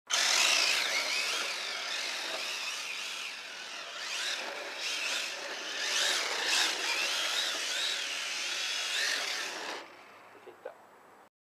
Traxxas Rally VXL Drift 2
This is a quick video of my traxxas rally VXL, I am using HPI T-Drift tyres. In this video I was using 2 x 3s Rhino 1550mah lipos in parallel.